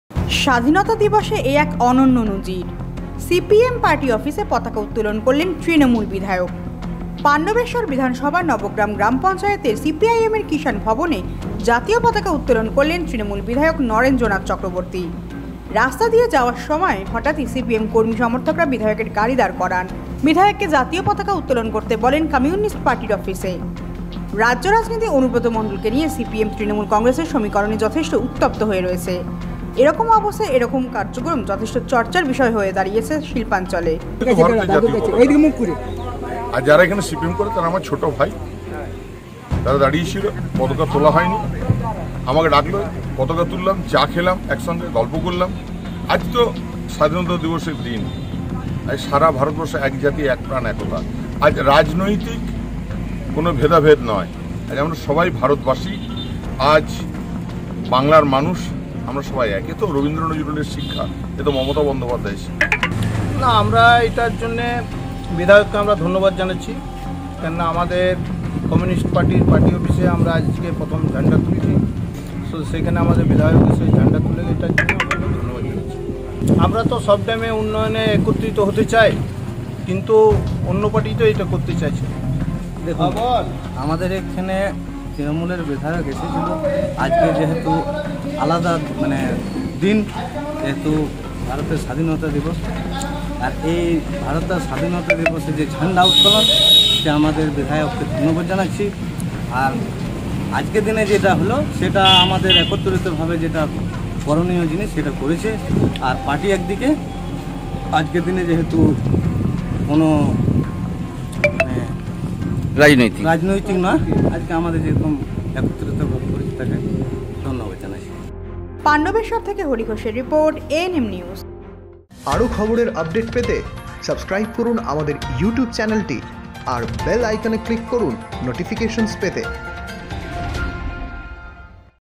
বিরোধী দলের পার্টি অফিসে পতাকা উত্তোলন করলেন তৃণমূল বিধায়ক/tmc MLA hoisted flag at cpim party office
হরি ঘোষ, পাণ্ডবেশ্বর : স্বাধীনতা দিবসে এ এক অনন্য নজির ! সিপিএম পার্টি অফিসে পতাকা উত্তোলন করলেন তৃণমূল বিধায়ক। পান্ডবেশ্বর বিধানসভার নবগ্রাম গ্রাম পঞ্চায়েতের সিপিআইএমের কিষান ভবনে জাতীয় পতাকা  উত্তোলন করলেন তৃণমূল বিধায়ক নরেন্দ্রনাথ চক্রবর্তী । রাস্তা দিয়ে যাওয়ার পথে হঠাৎই সিপিএম কর্মী সমর্থকরা বিধায়কের গাড়ি দাঁড় করান। এবং বিধায়ককে জাতীয় পতাকা উত্তোলন করতে বলেন কমিউনিস্ট পার্টির অফিসে। রাজ্য রাজনীতি অনুব্রত মণ্ডলকে নিয়ে সিপিএম এবং তৃণমূল কংগ্রেসের সমীকরণে যথেষ্ট উত্তাপ রয়েছে।এমতাবস্থায় এহেন কার্যক্রম যথেষ্ট  চর্চার বিষয় শিল্পাঞ্চলে।

এ বিষয়ে তৃণমূল বিধায়ক নরেন্দ্রনাথ চক্রবর্তী বলেন, 'আজকের এই শুভ স্বাধীনতা দিবসের প্রাক্কালে কোনো ব্যাপারে রাজনৈতিক রং দেওয়া উচিত নয় ।আমি যেহেতু এই  অঞ্চলের  বিধায়ক তাই তারা অনুরোধ করেন এবং সেই মোতাবেক আমি জাতীয় পতাকা  উত্তোলন করি।' কিন্তু মাননীয় বিধায়ক কিছুটা হলেও রাজনৈতিক রং চড়িয়ে বলেন, 'সিপিআইএম সরকার মাননীয়া মুখ্যমন্ত্রী মমতা বন্দ্যোপাধ্যায়কে তৎকালীন জ্যোতি বসুর সরকার রাইটার্স বিল্ডিং থেকে চুলের মুঠি ধরে বার করে দিয়েছিলেন ।কিন্তু ২০১১ সালে তৃণমূল কংগ্রেস সরকার গঠন করলে সেই কারাগারের নম্বরে সিপিএম নেতৃত্ব তথা বুদ্ধদেব ভট্টাচার্য কিন্তু সহ সিপিএম নেতৃত্বকে ফিসফ্রাই খাইয়েছিলেন।' অন্যদিকে, আজমপুর সিপিআইএমের নবগ্রাম গ্রাম পঞ্চায়েতের প্রাক্তন প্রধান কাঞ্চন মুখার্জি বলেন, 'মাননীয় বিধায়ক আমাদের ঘরের মানুষ। এ বিষয়ে কোনো রাজনৈতিক রং দেওয়া উচিত নয় ।তৃণমূল কংগ্রেসের  সাথে আমাদের রাজনৈতিক মতপার্থক্য থাকলেও এই স্বাধীনতা দিবসের প্রাক্কালে আমরা সকলে একত্রিত হয়েছি।